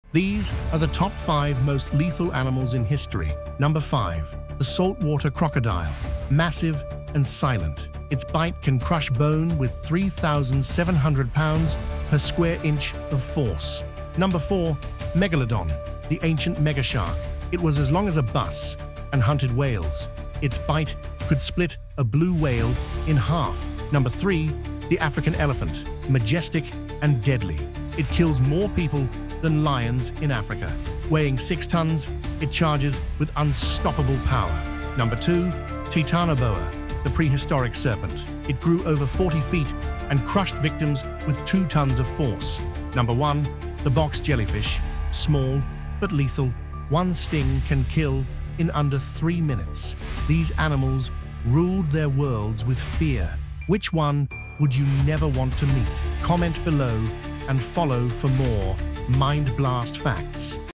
Top 5 Most Lethal Animals in History
What do a prehistoric snake, a modern jellyfish, and an ancient mega-shark have in common?
They’re all on this countdown of the Top 5 most lethal animals in history — creatures that ruled their worlds with fear and power.

From the terrifying Titanoboa to the silent sting of the box jellyfish, we blend history, science, and cinematic visuals to bring these killers to life.

🎥 Follow for more dark history and mythic facts — only on Mindblast.